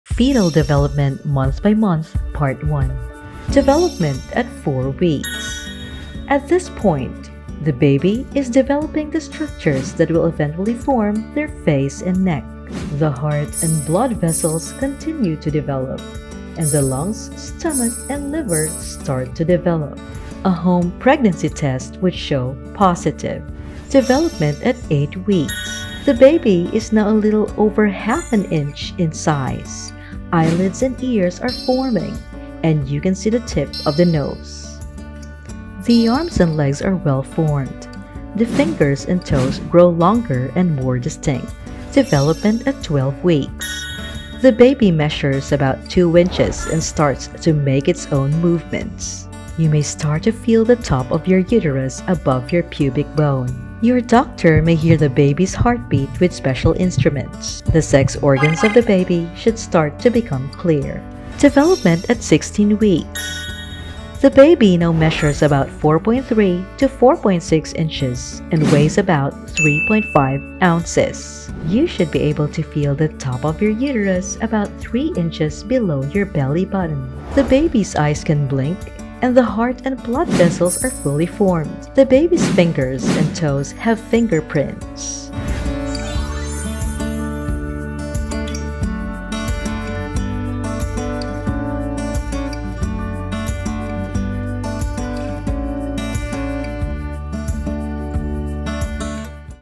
WATCH! Week by Week Fetal Development | Part 1
Wondering how babies grow inside your womb? Here's a video that shows how amazing your little one enjoys life, as he or she grows in your womb. WATCH THIS NOW